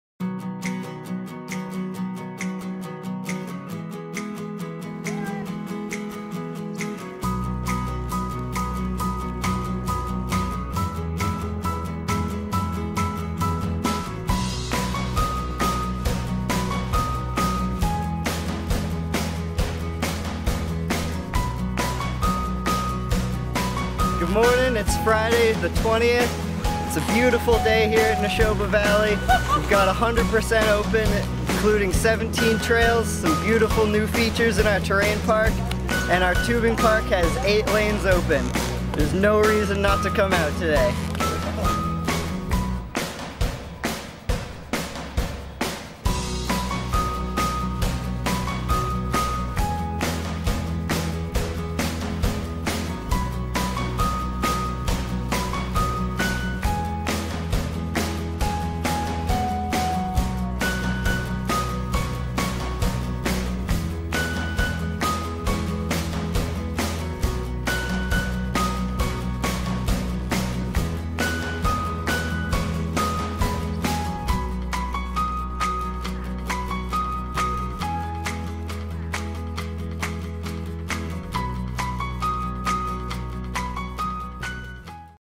December 20, 2013 Snow Report
Nashoba is 100% open today! We have 17 trails, 8 lifts, and 8 slow tubing park lanes. Also, our terrain park is in full swing with 12 features, 4 rails, and some huge jumps for you!